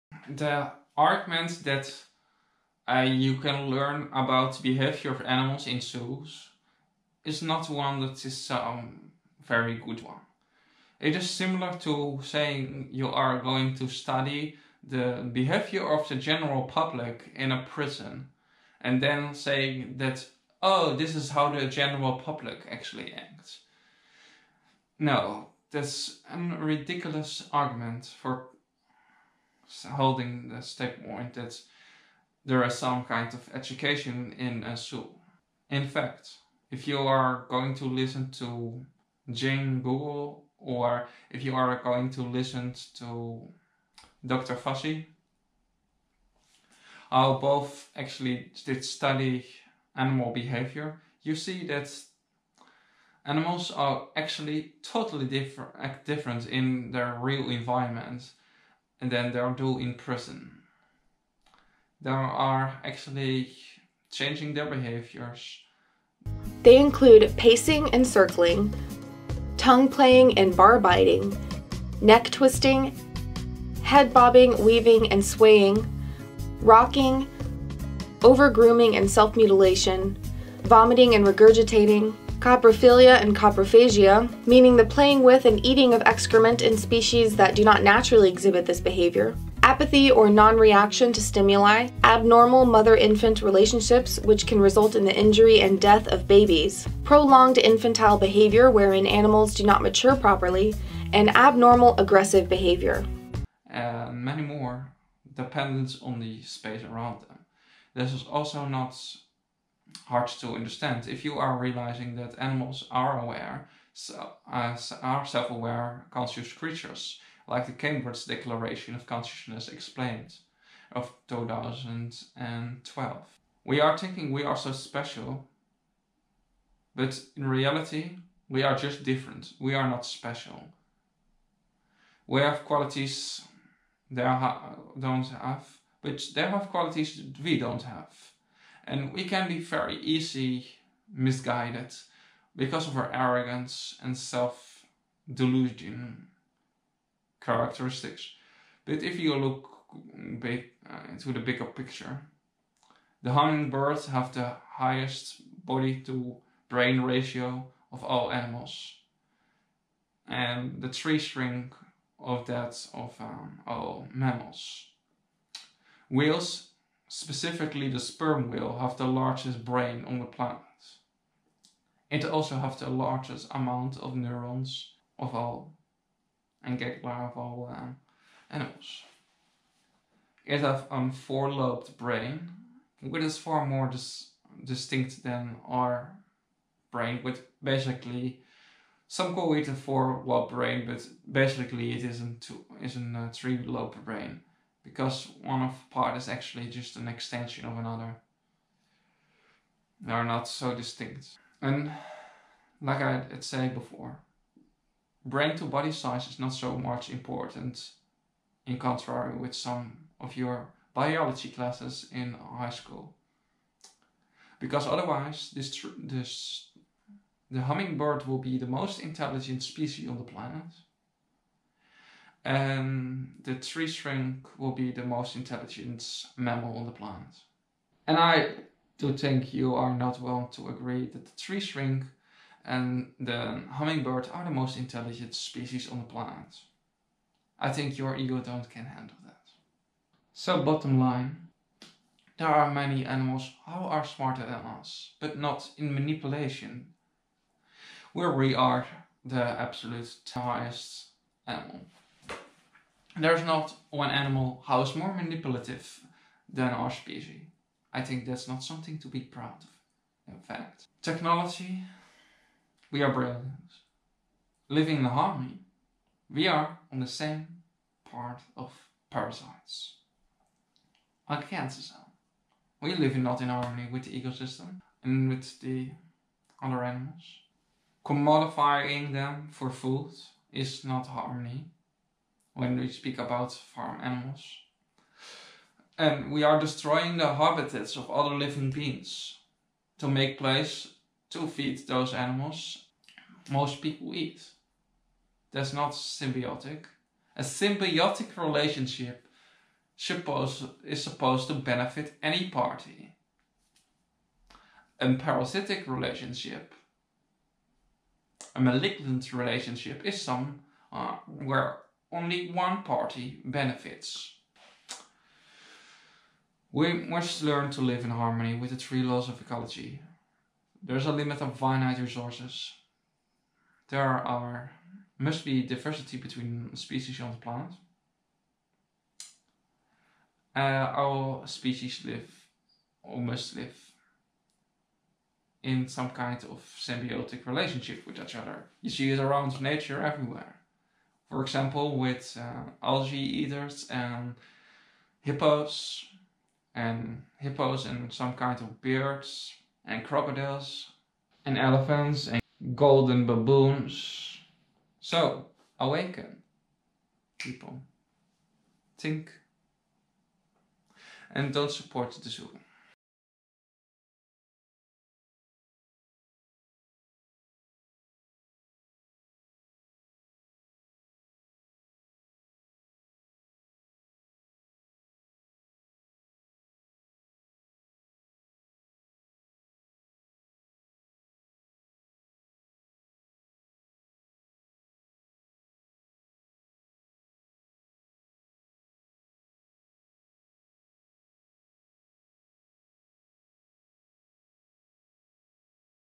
why zoo are not educational part 2
July 7, 2012, a prominent international group of cognitive neuroscientists,
neuropharmacologists, neurophysiologists, neuroanatomists and computational neuroscientists
gathered at The University of Cambridge to reassess the neurobiological substrates of conscious
experience and related behaviors in human and non-human animals. While comparative research on
this topic is naturally hampered by the inability of non-human animals, and often humans, to clearly
and readily communicate about their internal states, the following observations can be stated
unequivocally:

Zoos
7d hologram since 2016

Gorillas in the Mist by
dian fossey

Murders in the Mist: Who Killed Dian Fossey? By Nicholas Gordon